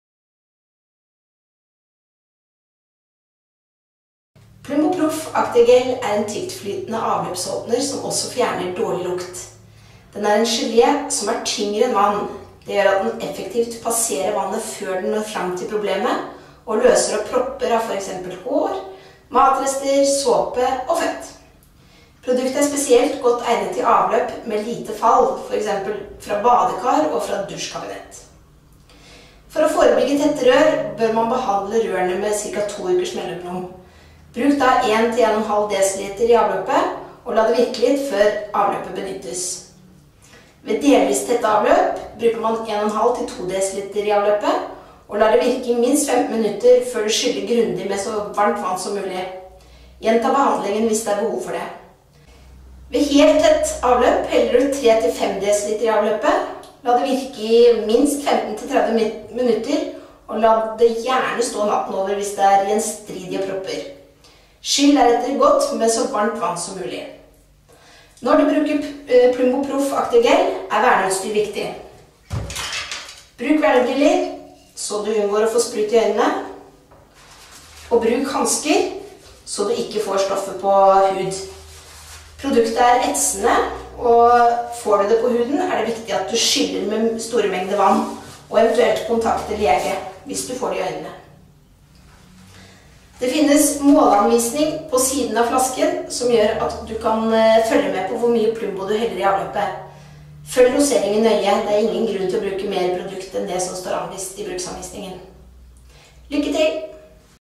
Plumbo Proff Aktiv Gel
Avløpsåpner til bruk i alle typer sluk og avløp. En tung gele som løser opp alle typer propper av biologisk materiele som fett, hår, såpe etc.